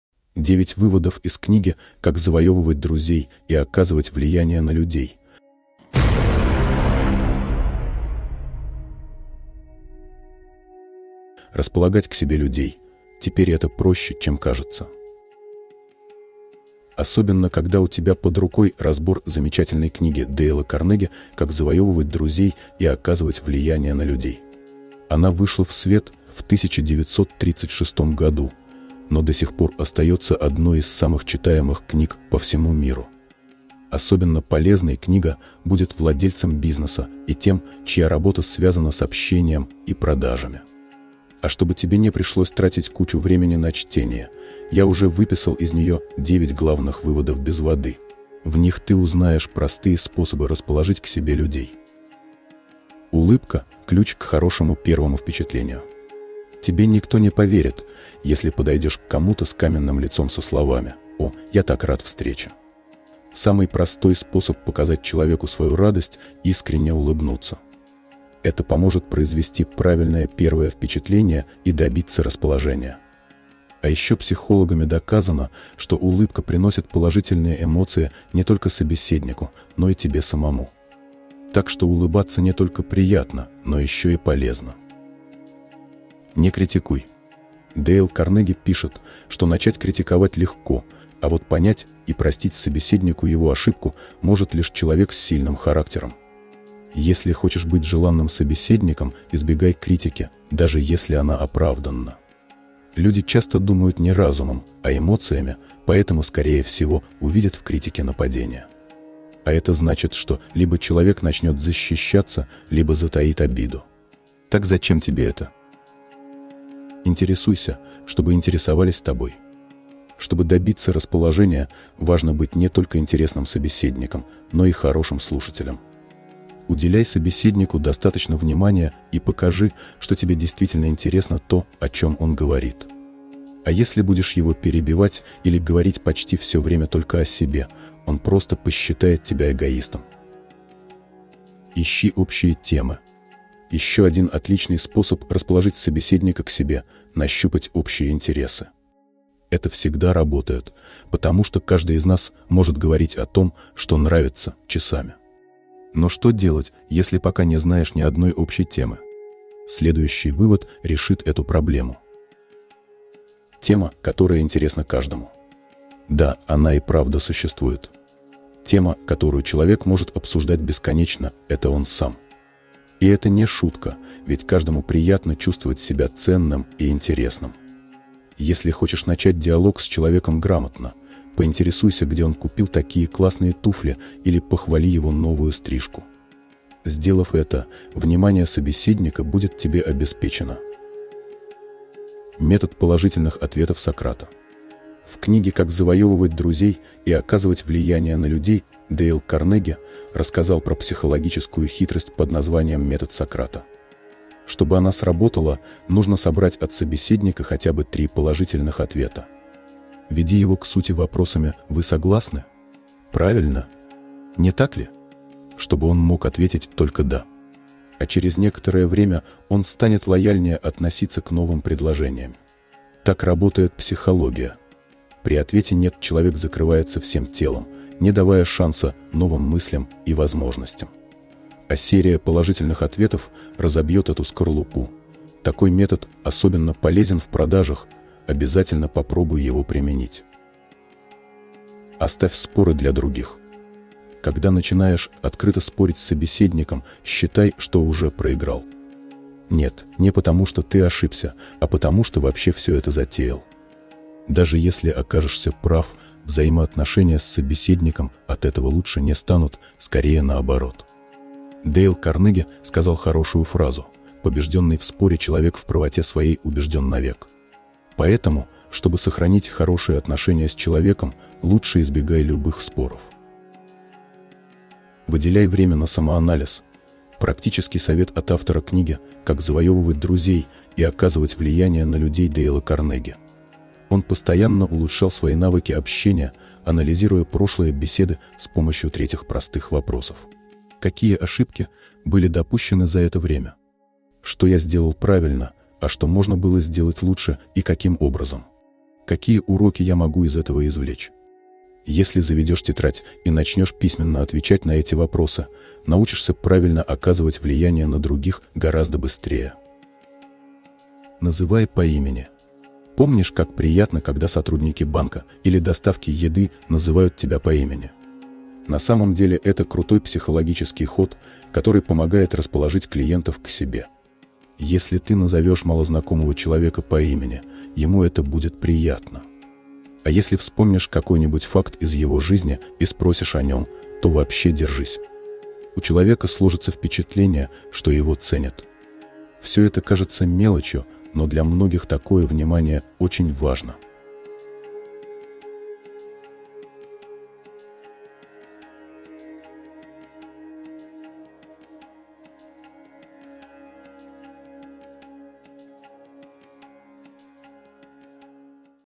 9 выводов из книги «Как завоёвывать друзей и оказывать влияние на людей»
Располагать к себе людей? Теперь это проще, чем кажется.

Особенно когда у тебя под рукой разбор замечательной книги Дейла Карнеги «Как завоёвывать друзей и оказывать влияние на людей». Она вышла в свет в 1936 году, но до сих пор остаётся одной из самых читаемых книг по всему миру!

Особенно полезной книга будет владельцам бизнеса и тем, чья работа связана с общением и продажами. А чтобы тебе не пришлось тратить кучу времени на чтение, я уже выписал из неё 9 главных выводов без воды. В них ты узнаешь простые способы расположить к себе людей.

Карта Сбербанка: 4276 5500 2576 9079
Карта Тинькофф: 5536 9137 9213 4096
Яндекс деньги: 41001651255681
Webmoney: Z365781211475